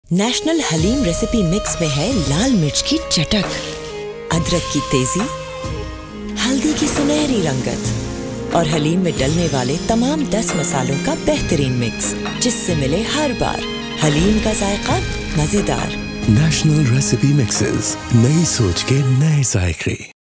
National Recipe Mixes - Haleem
Straight from National's recipe treasure comes National Haleem Recipe Mix & Quick cook Haleem, with ten essential spices that give a homely touch to your favorite piping hot Haleem.